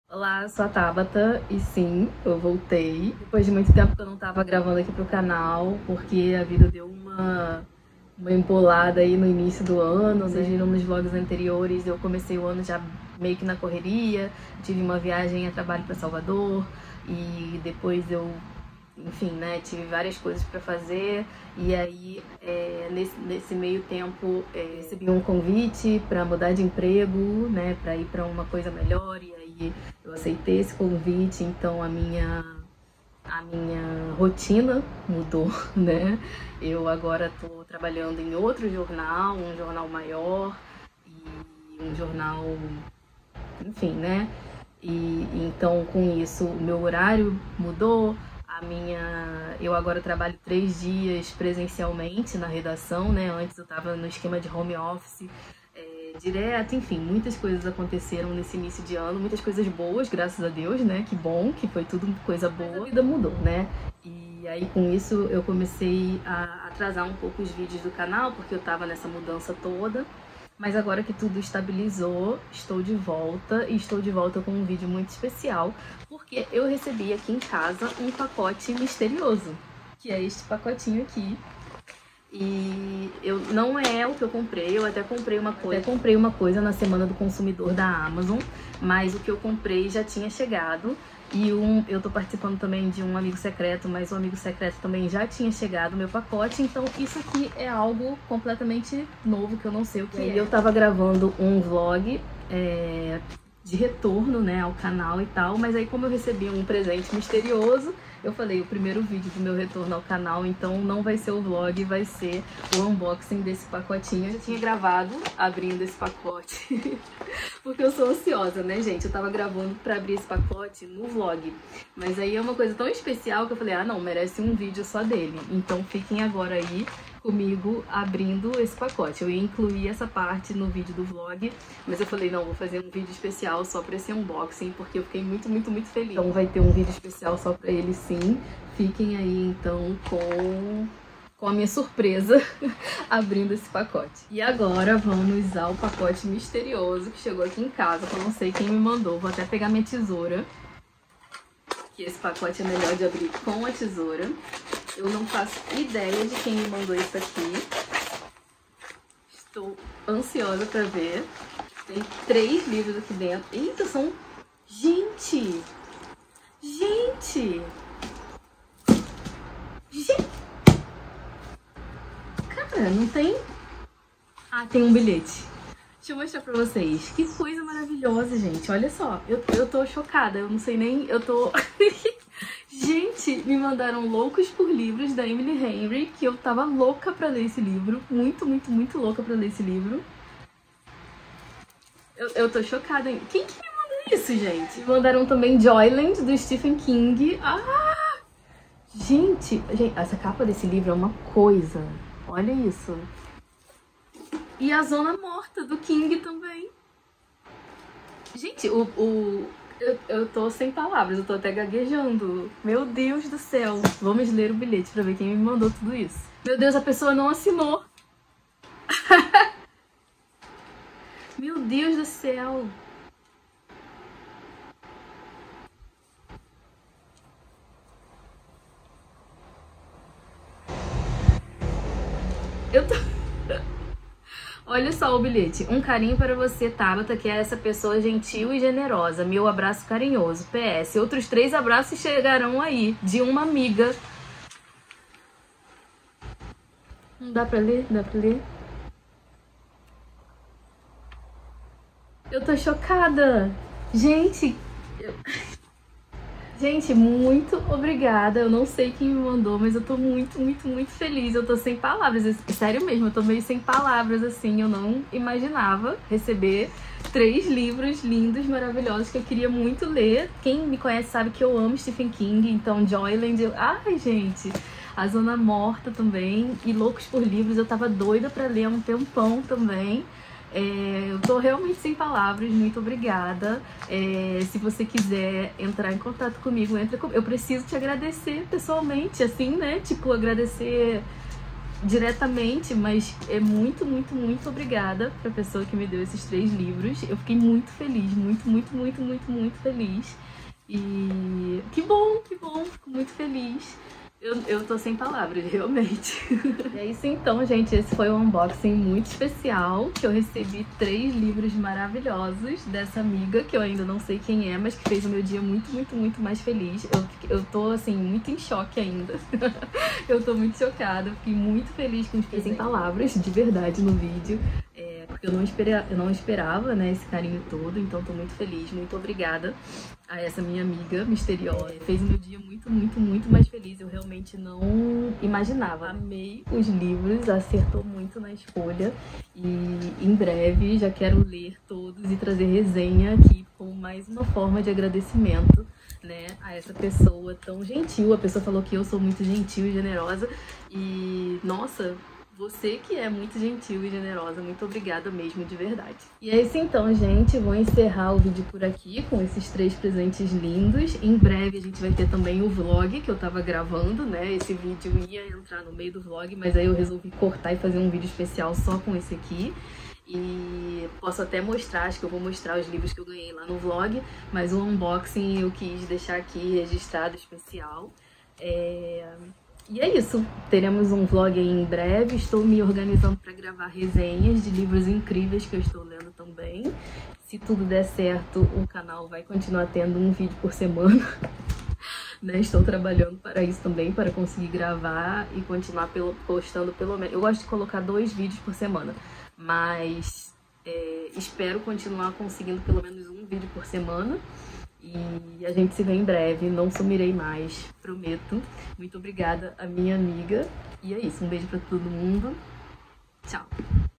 UNBOXING MISTERIOSO E MUITO ESPECIAL ❤️✨
Olá, pessoal! Neste vídeo eu mostro os livros que eu recebi de uma amiga muito especial. Espero que gostem!

Minha lista de desejos na Amazon